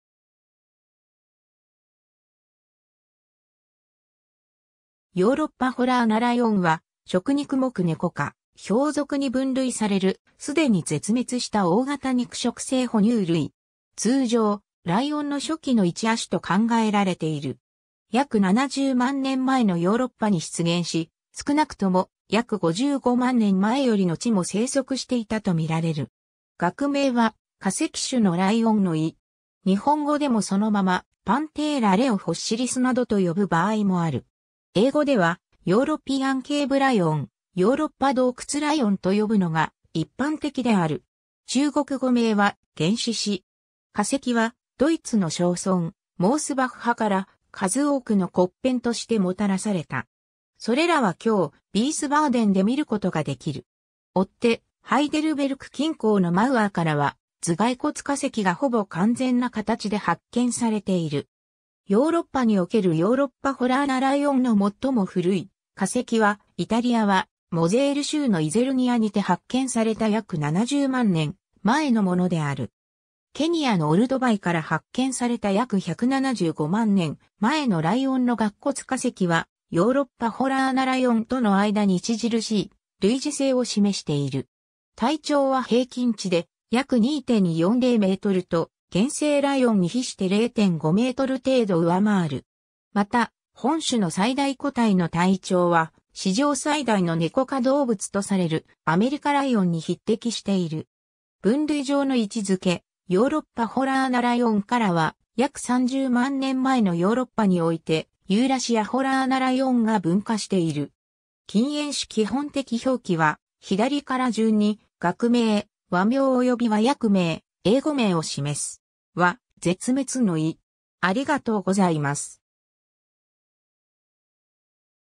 ヨーロッパホラアナライオン
ヨーロッパホラアナライオン（学名：Panthera leo fossilis）は、食肉目（ネコ目）- ネコ科- ヒョウ属に分類される、すでに絶滅した大型肉食性哺乳類。
通常、ライオンの初期の1亜種と考えられている。
 約70万年前（新生代第四紀更新世チバニアン）のヨーロッパに出現し、少なくとも約55万年前（同世同期）よりのちも棲息していたと見られる。
 学名は、「化石種（fossilis）のライオン（Panthera leo）」の意。
 日本語でもそのまま「パンテーラ・レオ・フォッシリス」などと呼ぶ場合もある。
 英語では European cave lion 「ヨーロッパ洞窟ライオン」と呼ぶのが一般的である。
 中国語名は「原始獅」（yuánshǐshī; ユアンシーシー）。
 化石は、ドイツの小村モースバッハ（Mosbach。）から数多くの骨片としてもたらされた。
それらは今日、ヴィースバーデンで見ることができる。
 追って、ハイデルベルク近郊のマウアーからは頭蓋骨化石がほぼ完全な形で発見されている。
 ヨーロッパにおけるヨーロッパホラアナライオンの最も古い化石は、イタリアはモゼール州のイゼルニアにて発見された約70万年前のものである。
 ケニアのオルドヴァイから発見された約175万年前（更新世初期）のライオンの顎骨化石は、ヨーロッパホラアナライオンとの間に著しい類似性を示している。
 体長は平均値で約2.40 mと、現生ライオンに比して0.5m程度上回る。
 また、本種の最大個体の体長は、史上最大のネコ科動物とされるアメリカライオン（学名：Panthera atrox、英語名：American Lion。更新世前中期。絶滅種）に匹敵している。
 分類上の位置づけ ヨーロッパホラアナライオンからは、約30万年前（更新世後期）のヨーロッパにおいて、ユーラシアホラアナライオン（Panthera spelaea、英語名：Cave lion、Upper Pleistocene European cave lion 〈後者は「更新世後期ヨーロッパ洞窟ライオン」の意〉）が分化している。
 近縁種 基本的表記は左から順に、学名、和名および和訳名、英語名を示す。
† は「絶滅」の意。